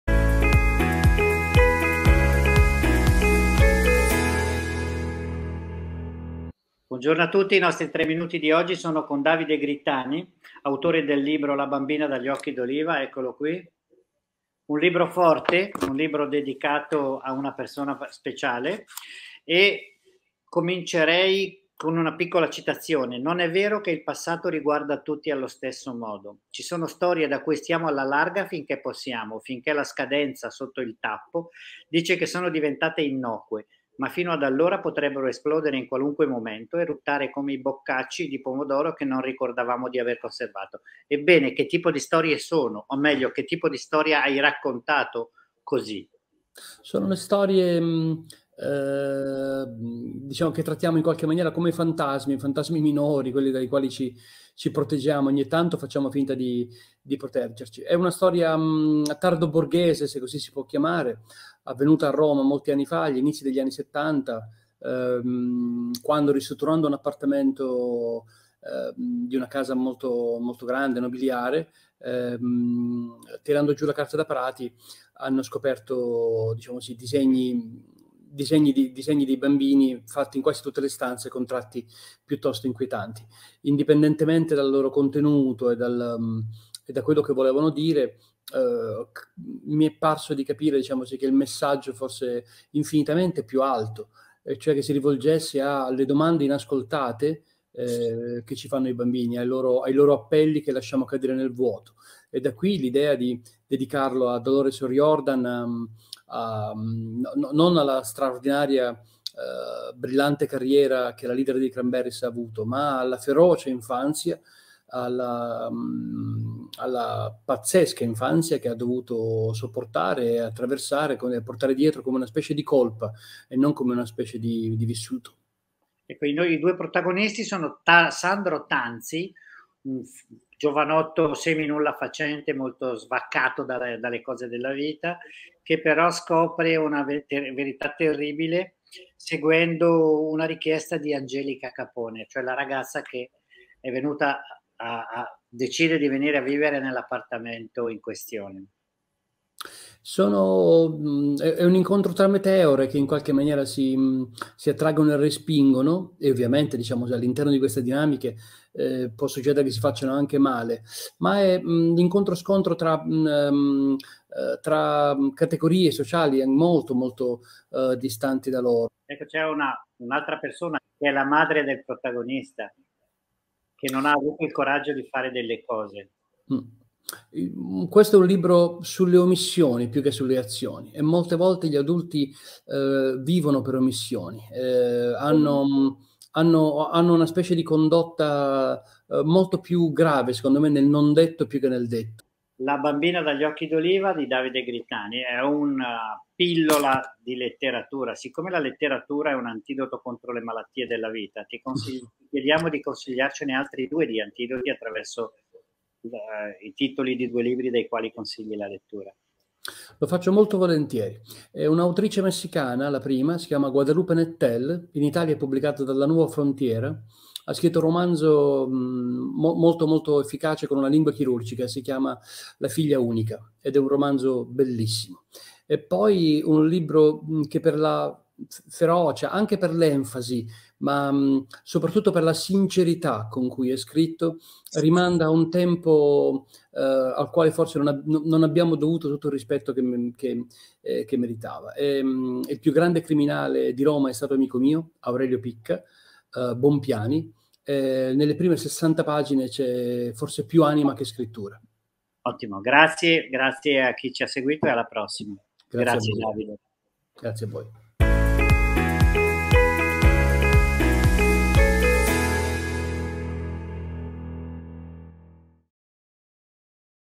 3 minuti 1 libro: Davide Grittani presenta "La bambina dagli occhi d'oliva"
Davide Grittani presenta "La bambina dagli occhi d'oliva", tratto da un fatto realmente accaduto a Roma negli anni '70. Una cupa storia di segreti svelati e differenze sociali, riassunta in un'intervista curata da Paolo Gualandris.